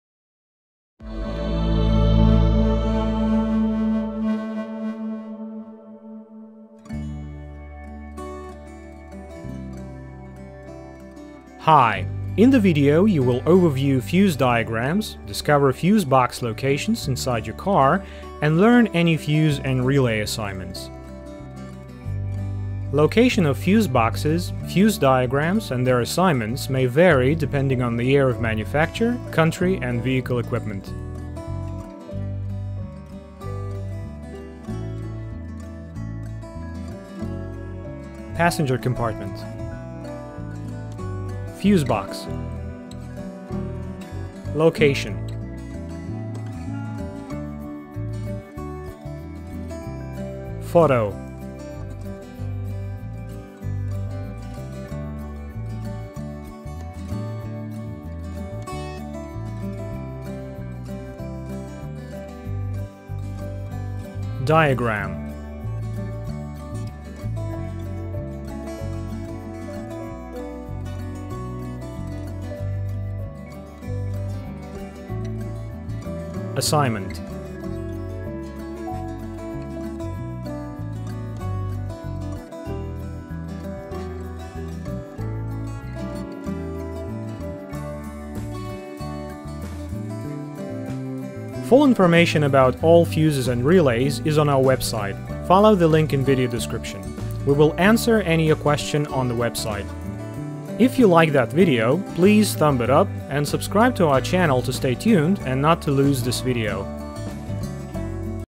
Fuse box diagram Ford Expedition 2003 - 2006 and relay with assignment and location
Description of the designation of fuses and relays Ford Expedition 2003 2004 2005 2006  with a box diagram and a photo example. Cigarette lighter fuse.